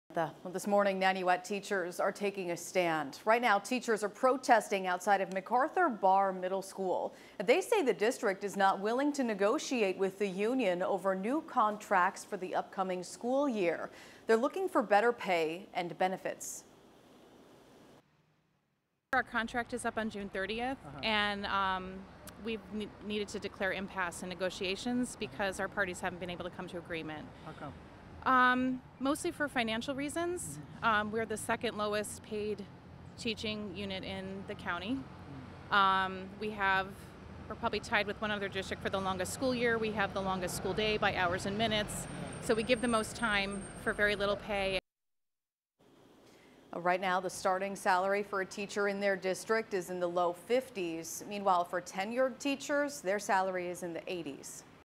Nanuet teacher protest low pay, long hours
Teachers gathered outside of MacArthur Barr Middle School claiming the school district is not willing to negotiate with the union over new contracts for the upcoming school year.